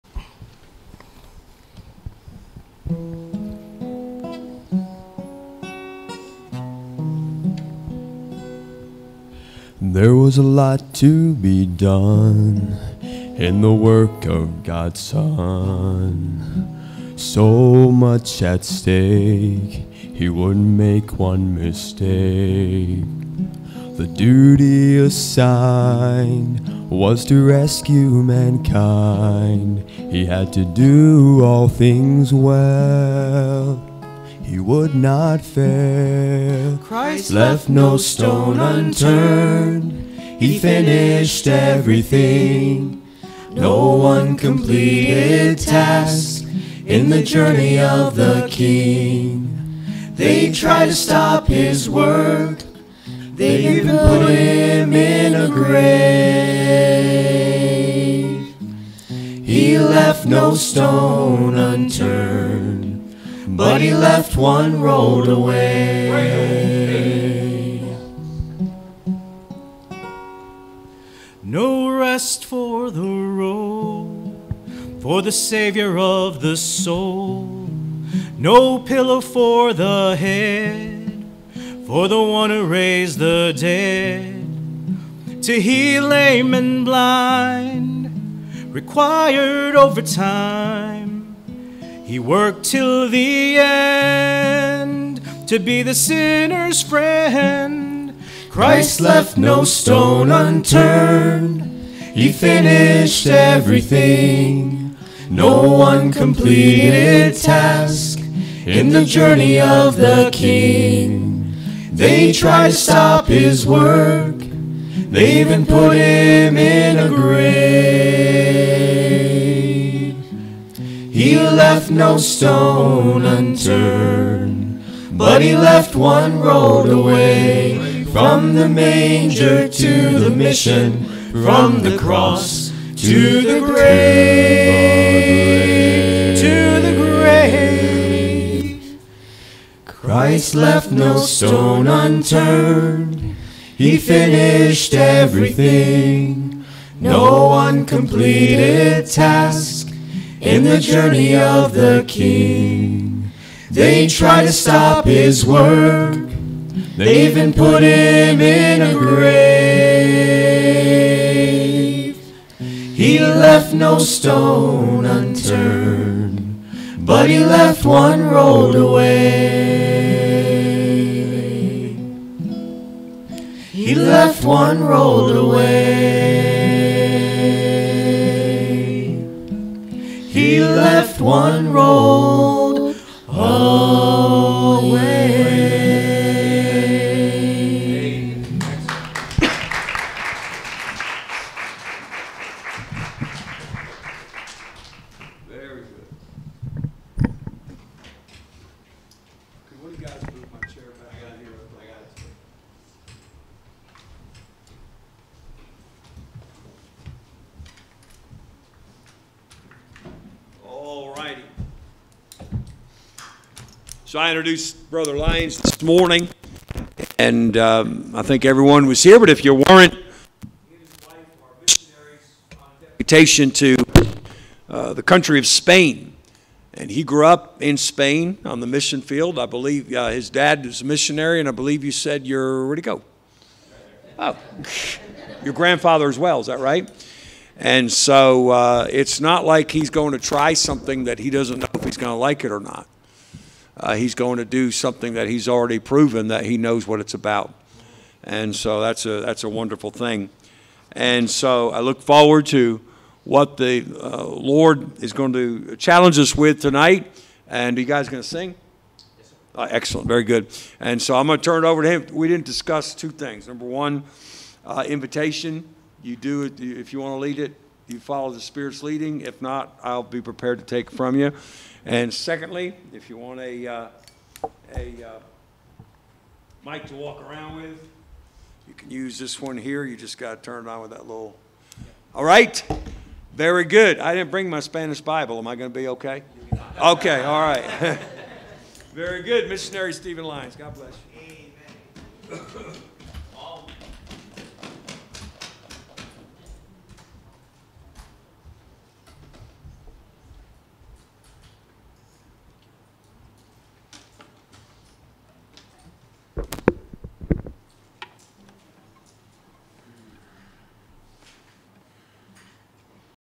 Sunday Evening Special Music -Men's Ensemble -09.01.24
Northeast Baptist Church
Sunday Evening Service
Special Music Clip
 Jose DeJesus, Jared Westbrook, Steven Vazquez  & Freddy Bambico